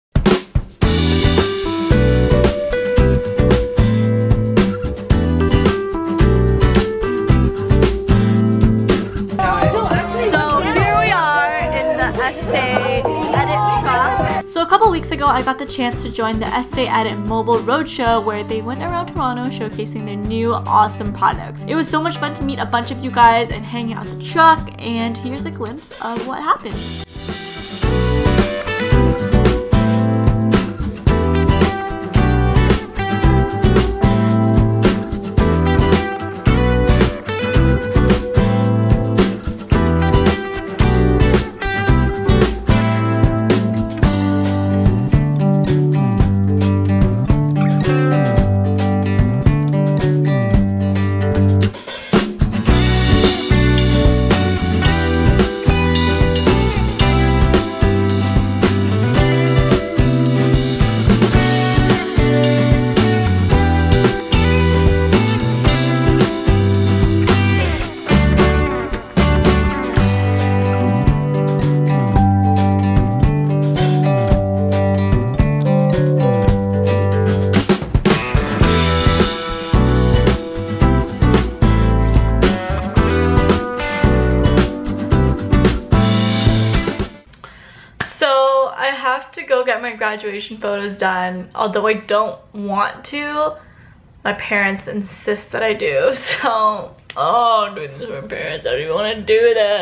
VLOG: Makeup In A Truck?! || The Estee Edit Roadshow Recap
I had the opportunity to hang out with you guys (in the freezing cold) to explore this new makeup line that's in Sephora's now! Such awesome products to check out - some of my faves include the Pore Stick and the Turn Up & Down lipsticks!